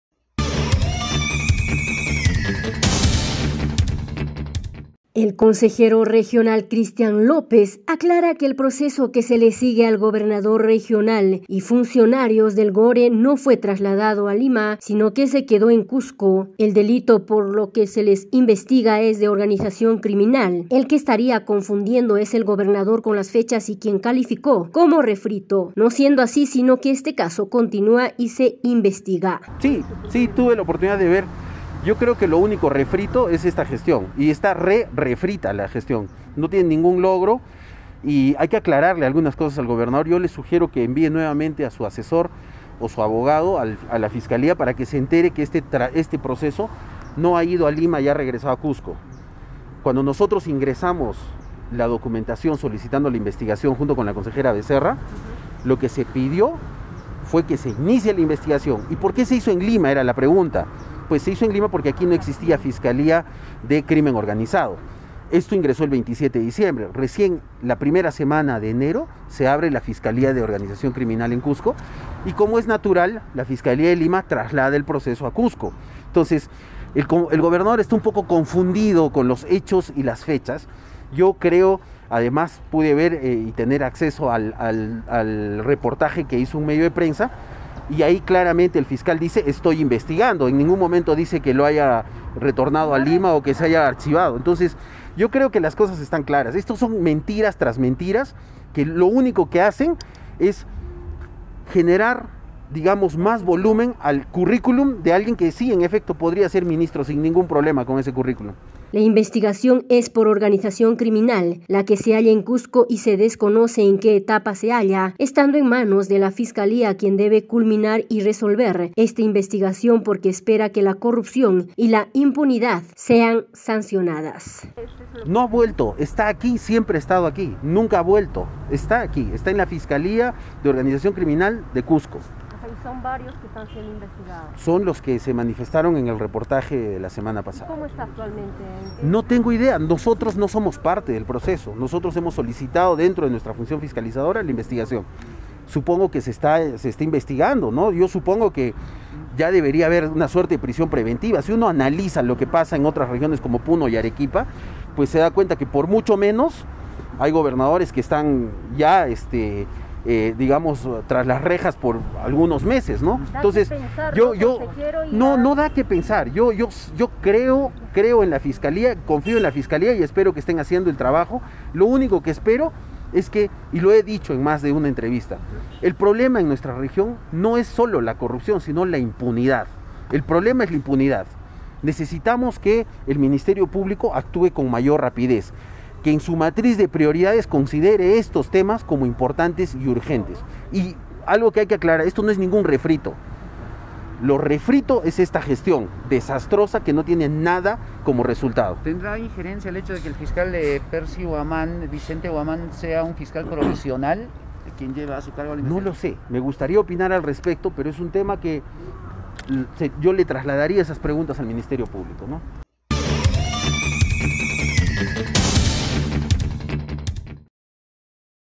CONSEJERO REGIONAL POR CUSCO CHRISTIAN LÓPEZ
MP FORMALIZÓ DENUNCIA CONTRA EL GOBERNADOR Y 35 FUNCIONARIOS TRAS HALLAR INDICIOS DE CORRUPCIÓN
La investigación al Gobernador Regional y 35 funcionarios del GORE por una organización criminal se realizará en Cusco motivo por el cual el Ministerio Público de la región ya formalizó la denuncia. El Consejero Regional Cristian López lamentó que el Gobernador intente minimizar esta determinación de la instancia judicial al hallar serios indicios de presuntos delitos cometidos.